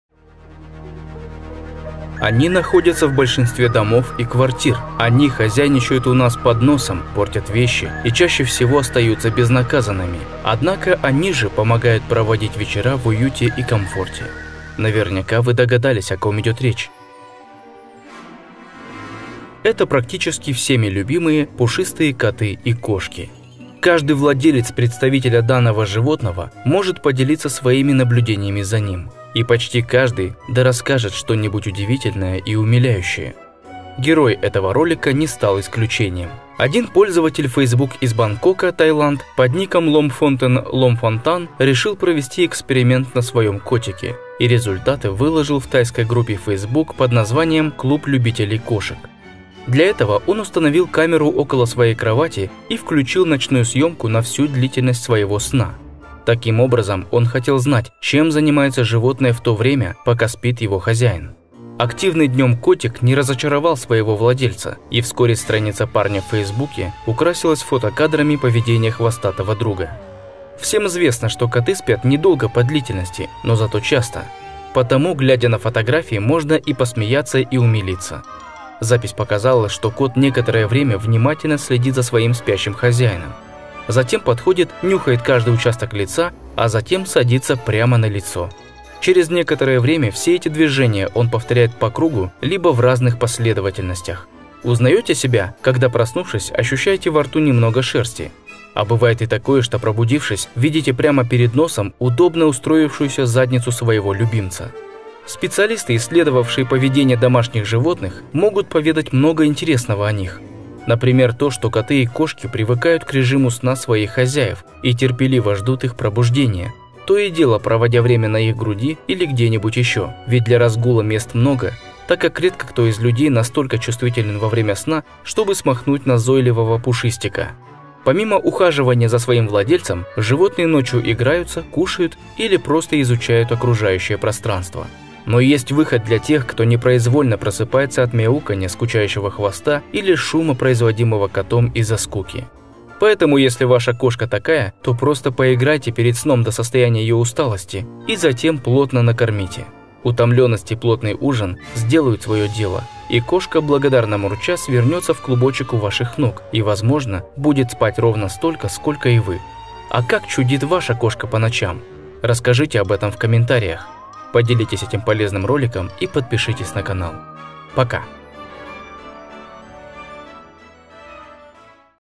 Мужчина устанавливает камеру, чтобы увидеть, что делает его кот пока он спит и это весело!
Каждый владелец котов и кошечек может поделиться своими наблюдениями за ними. И почти каждый да расскажет что-нибудь удивительное и умиляющее. Герой этого ролика не стал исключением и поделился ночной жизнью своего кота в соц. сетях.